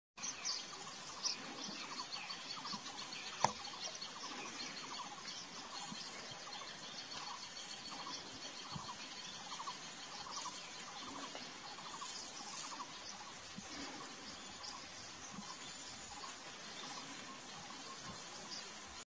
Rufous necked hornbill
Rufous necked hornbill recorded at Latpanchar